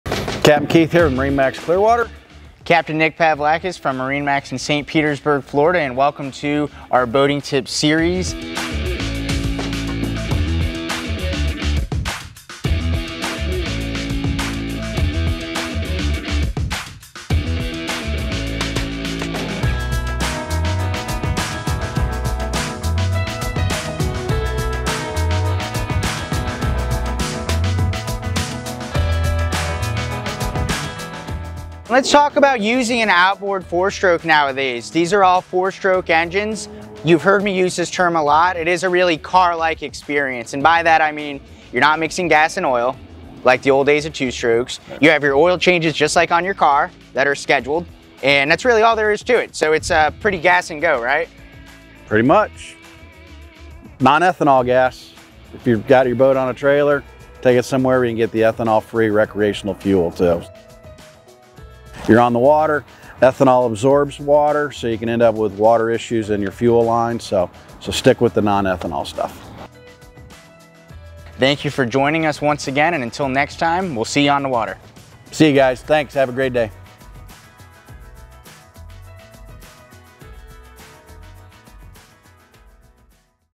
Boating Tips: Modern Engines and Boat Fuel
Captains Keith and Nick are talking marine fuel this week - what the proper fuel is, what not to do and why.